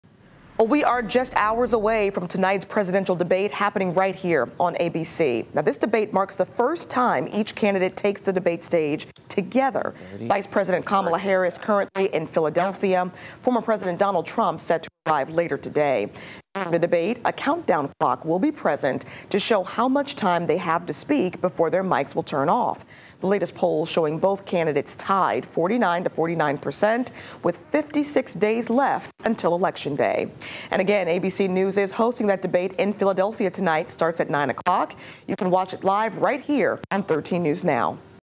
Presidential debate kicks off tonight
We are just hours away from tonight's presidential debate on ABC. This debate marks the first time each candidate will take the debate stage together. Vice President Kamala Harris is currently in Philadelphia. Former President Donald Trump is set to arrive later today. During the debate, a countdown clock will be present to show how much time they have to speak before their mics will turn off. The latest polls show both candidates tied 49 to 49 with 56 days left until election day.